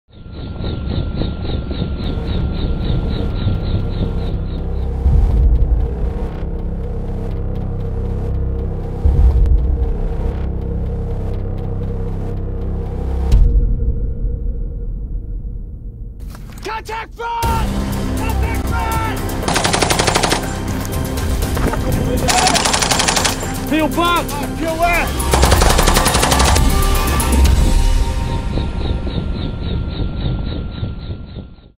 U.S. and Royal Marines - Brothers In Action
U.S. Marine Corps and British Royal Marine Commandos performing joint day and night operations at the Spainish Legion base in Almeria, Spain. Combining air and land assets, the Royal Marine Commandos and the U.S. Marine Corps deployed together before forming two opposing sides during a training exercise designed to test all aspects of attacking and defending positions in difficult terrain.